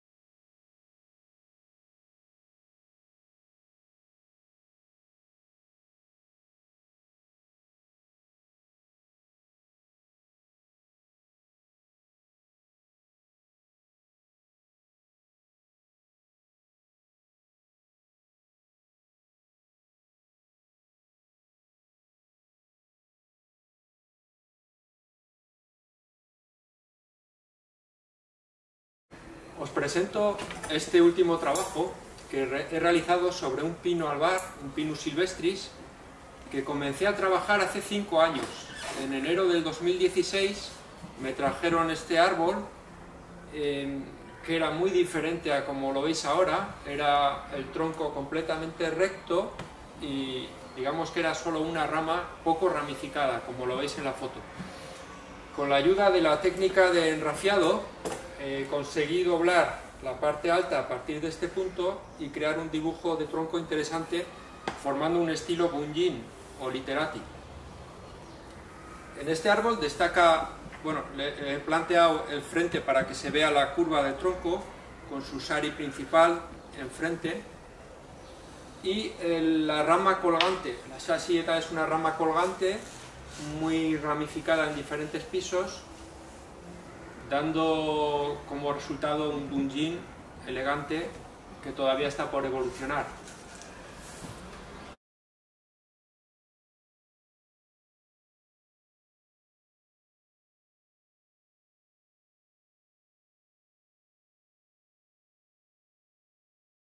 Pinus sylvestris bunjin 20/10/2021
Creación de un bonsái en estilo Bunjin
Pinus sylvestris